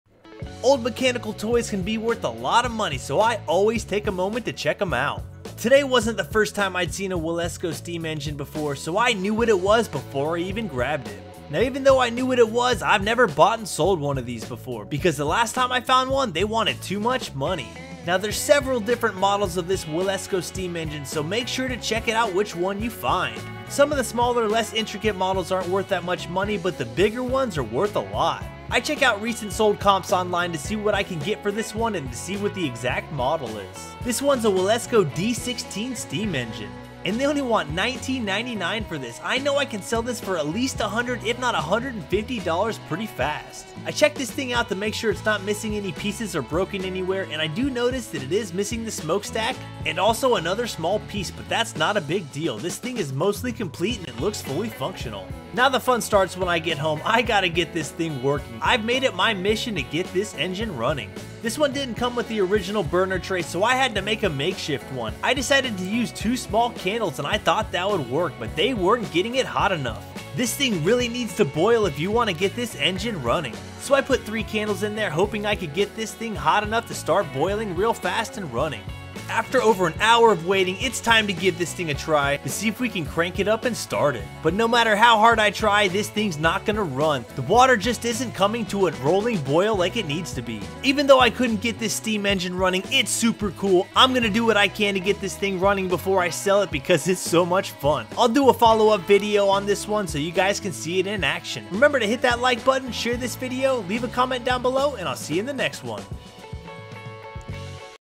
⚙️Old Mechanical Toys Sell EASY! Found a Wilesco Steam Engine Model D16 that is TOTALLY AWESOME!🔥
⚙️Old Mechanical Toys Sell EASY! Found a Wilesco Steam Engine Model D16 that is TOTALLY AWESOME!🔥Time to get this engine running again and have some fun!🔧